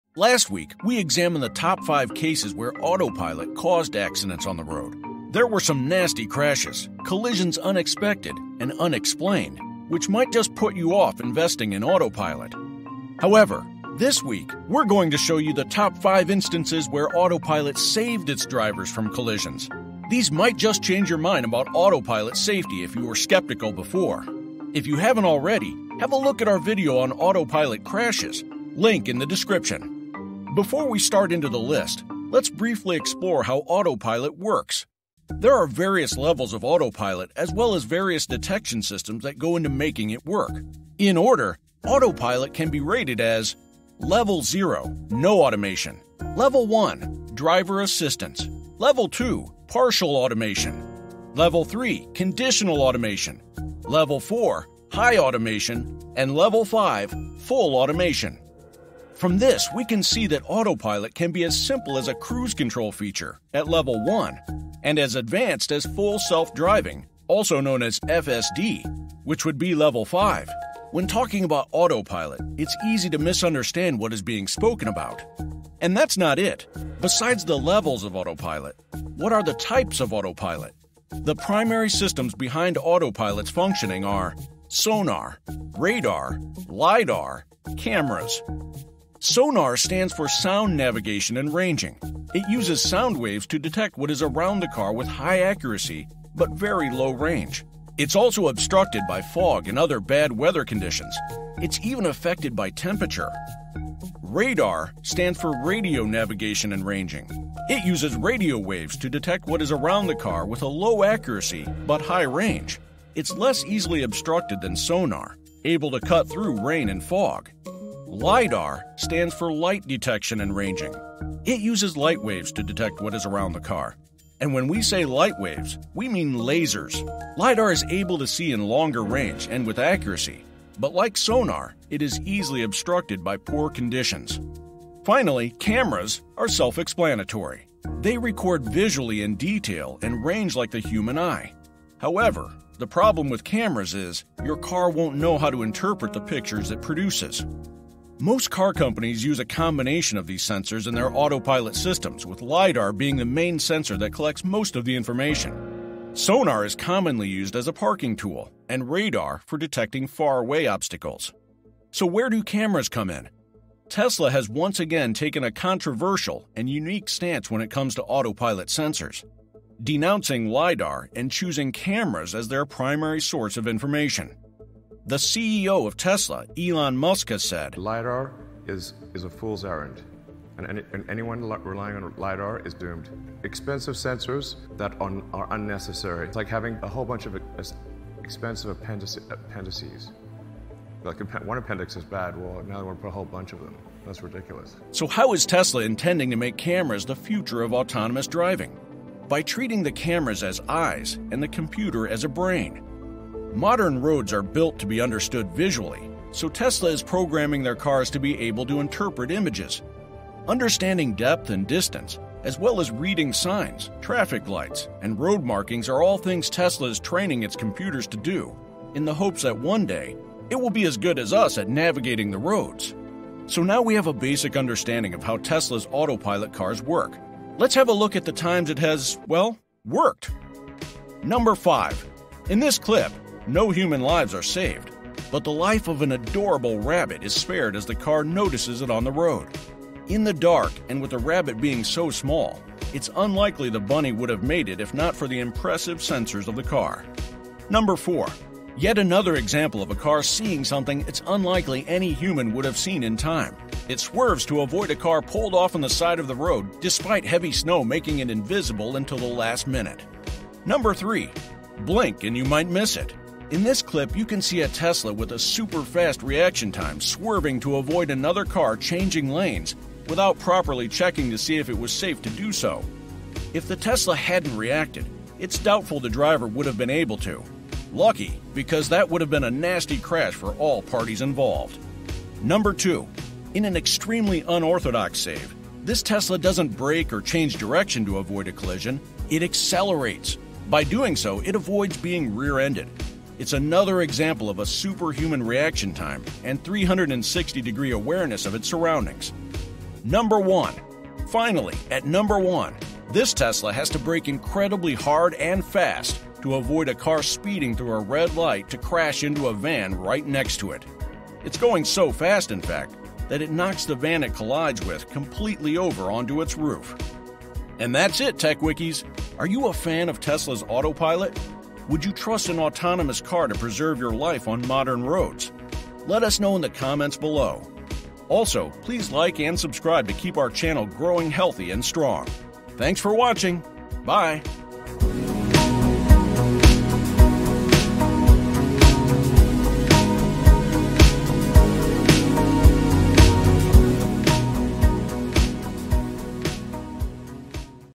Top TESLA Autopilot Saves || How Tesla Autopilot Work?
Last week we examined the top 5 cases where Autopilot caused accidents on the road. There were some nasty crashes - collisions unexpected and unexplained which might just put you off investing in autopilot. However, this week we’re going to show the top 5 instances where autopilot saved  its drivers from collisions. These might just change your mind about autopilot safety if you were skeptical before.
If you haven’t already, have a look at our video on autopilot crashes.
How safe is tesla?
Top 5 Tesla Autopilot Saves
How Tesla Autopilot Works?